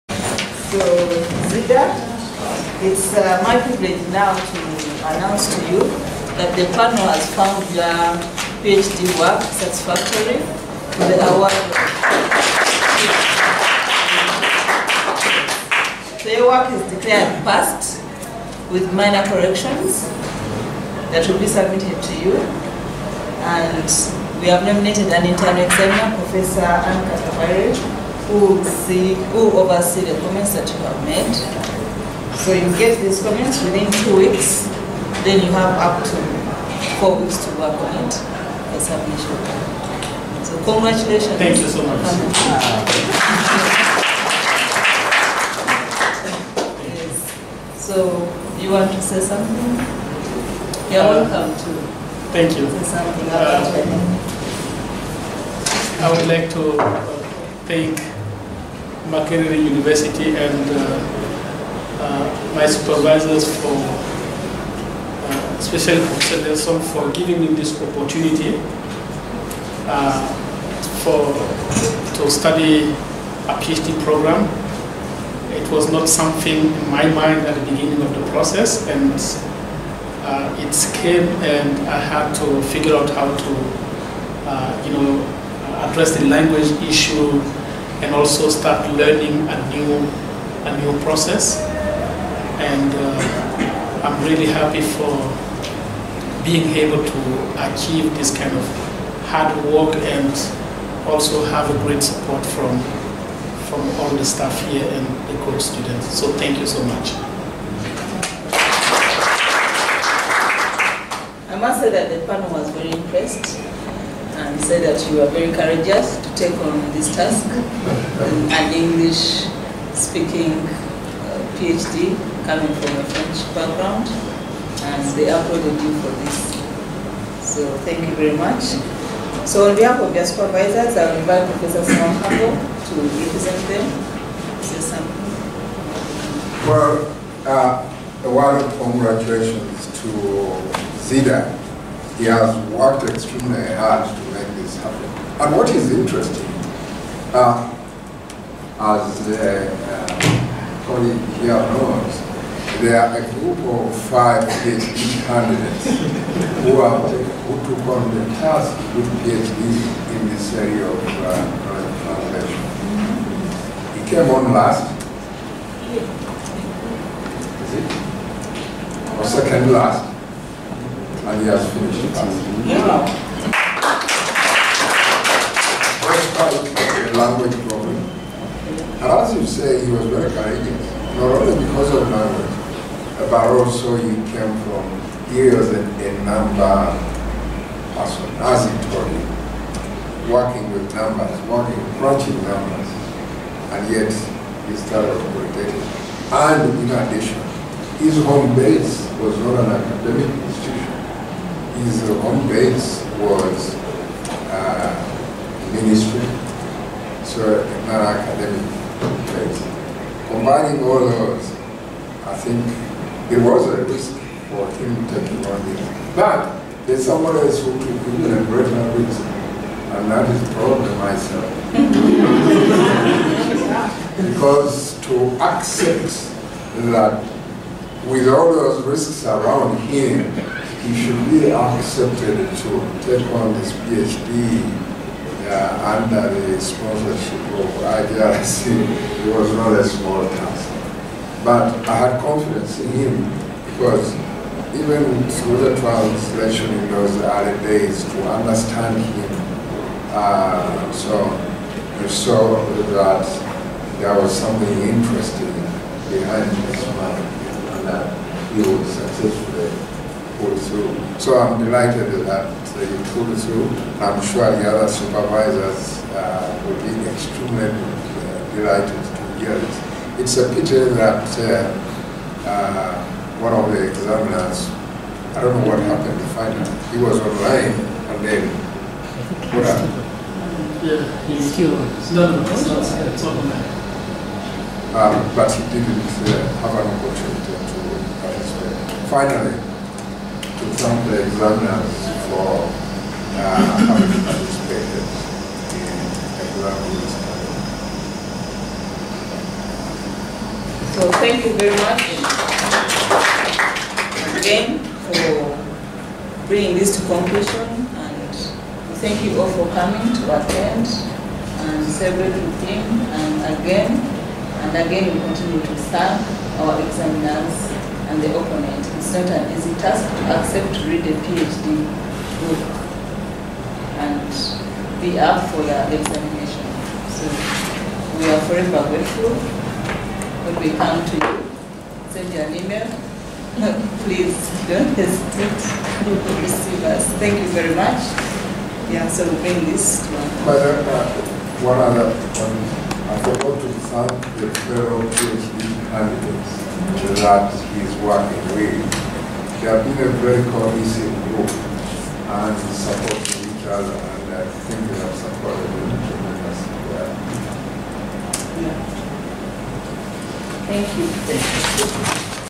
Results Announcement
Zida Andre: PHD Defense
Tittle: Creating and institutionalizing supports for evidence-informed decision making, including a rapid response service, in the Burkina Faso health system